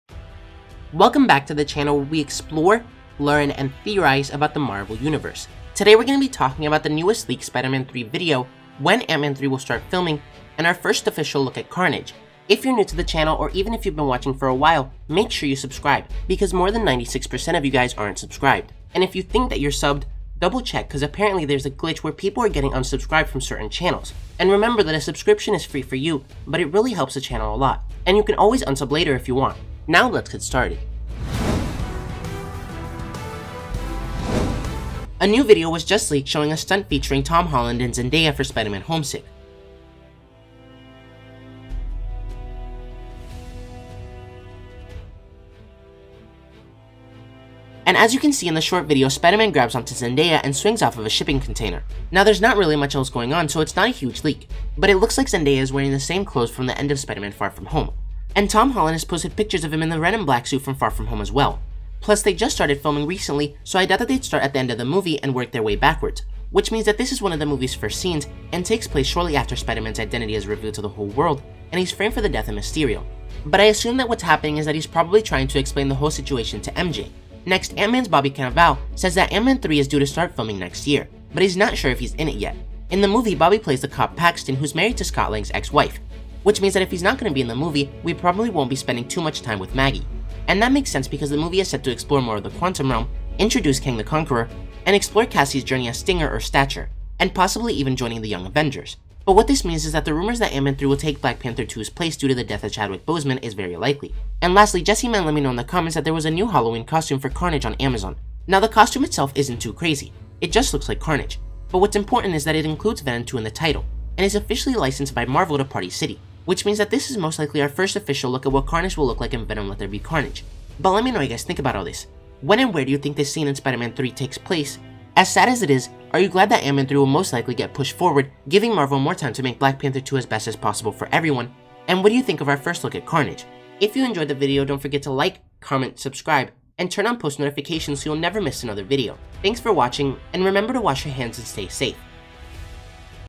First Official Look at Carnage in Venom 2, Spider-Man Homesick Leaked Footage, Ant-Man 3 Film Date
A new video was just leaked showing a stunt featuring Tom Holland and Zendaya for Spider-Man Homesick. As you can see in the short video Spider-Man grabs on to Zendaya and swings off of a shipping container. Now there’s not really much else going on so it’s not a huge leak but it looks like Zendaya is wearing the same clothes from the end of Spider-Man Far From Home, and Tom Holland has posted pictures of him in the red and black suit from Far From Home, plus they just started filming recently so I doubt they’d start from the end of the movie and work their way back which means that this is one of the movie’s first scenes and takes place shortly after Spider-Man’s identity is revealed to the whole world and he’s framed for the death of Mysterio. But I assume that he’s probably trying to explain the whole situation to MJ. Next, Ant-Man's Bobby Cannavale says that 'Ant-Man 3' is due to start filming next year — But he's not sure if he's in it yet. In the movie, Bobby plays the cop Paxton who’s married to Scott Lang’s ex-wife which means that if he’s not in the movie we probably won’t be spending too much time with Maggie, and that makes sense because the movie is said to explore more of the Quantum Realm, introduce Kang the Conqueror, and explore Cassie’s journey as Stinger/ Stature, and possibly even joining the Young Avengers. But what this means is that the rumors that Ant-Man 3 will take Black Panther 2’s place due to the death of Chadwick Boseman is very likely. Now the costume itself isn’t too crazy it just looks like Carnage, but what’s important is that it includes Venom 2 in the title and is officially licensed by Marvel to Party City, which means that this is most likely our first official look at what Carnage will look like in Venom Let There Be Carnage.